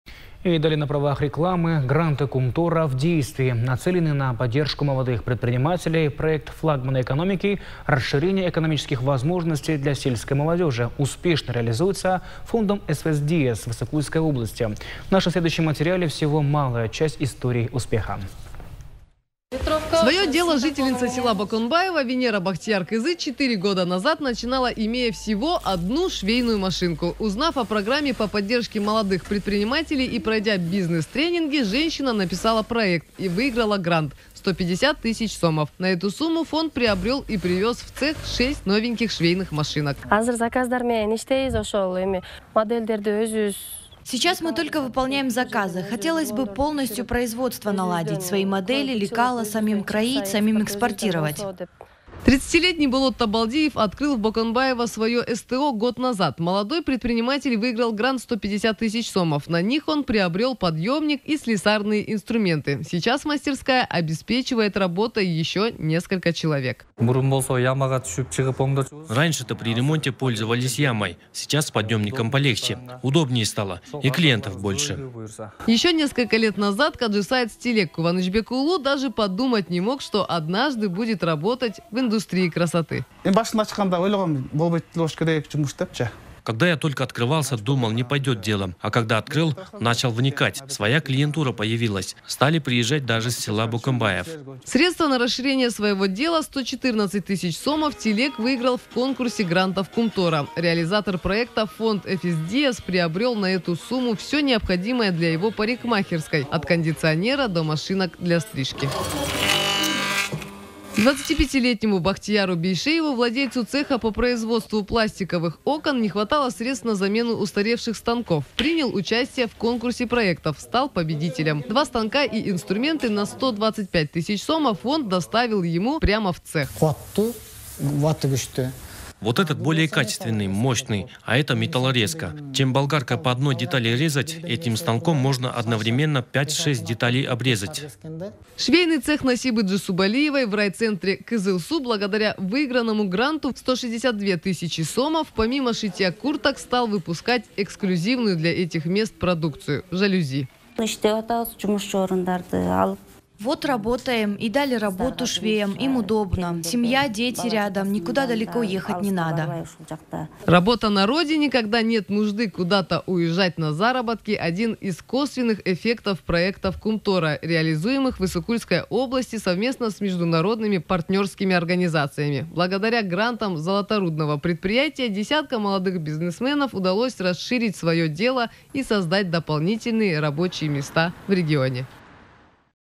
Гранты Кумтора в действии.
Нацеленный на поддержку молодых предпринимателей, проект Кумтор Голд Компани, «Расширение экономических возможностей для сельской молодежи», успешно реализуется фондом FSDS в Иссык-кульской области.